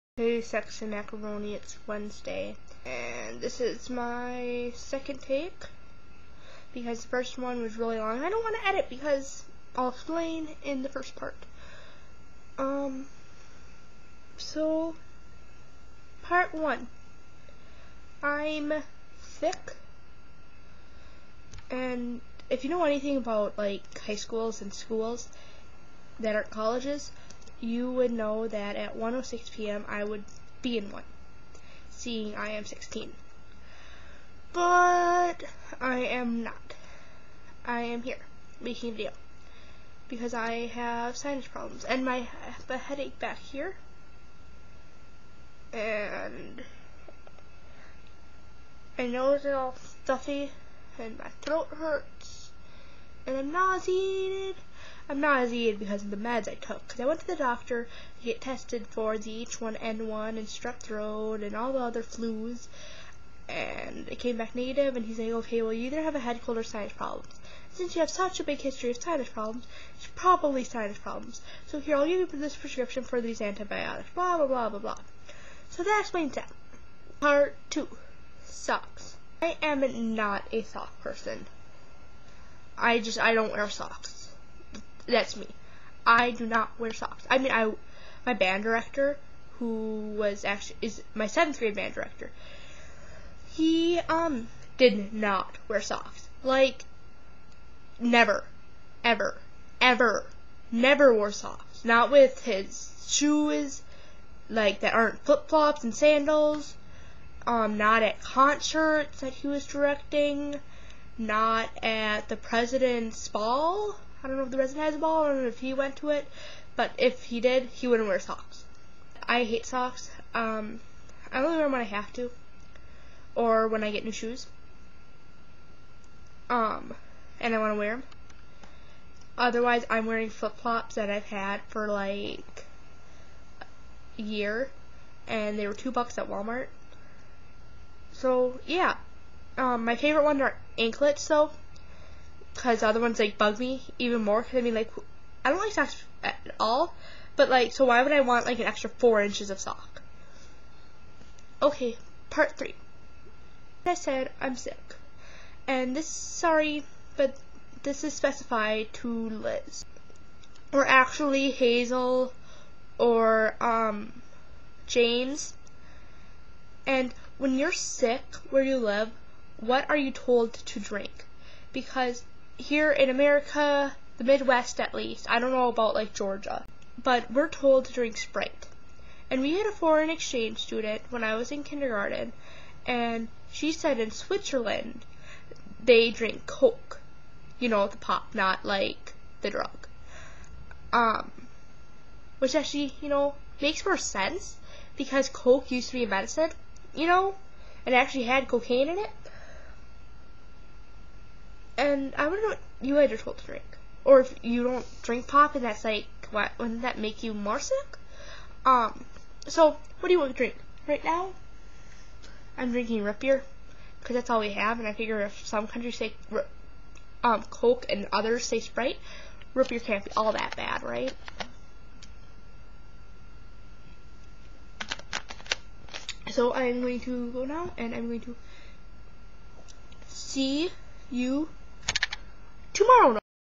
9/23: Marina does NOT have H1N1.
Next week, I can not complain in my video.
 I always complain in them.
 If I complain, you guys punish me. K?